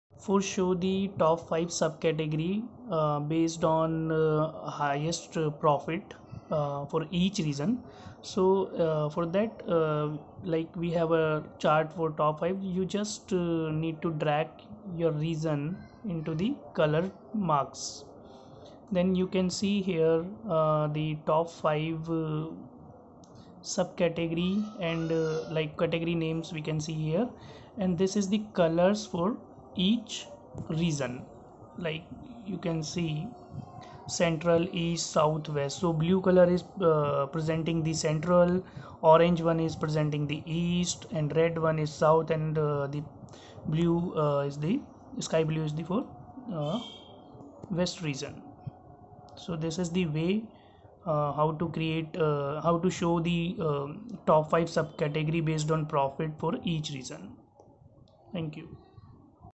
Tableau tutorial - Top N Sub category by Region
How to create chart for top 5 sub category based on profit for each region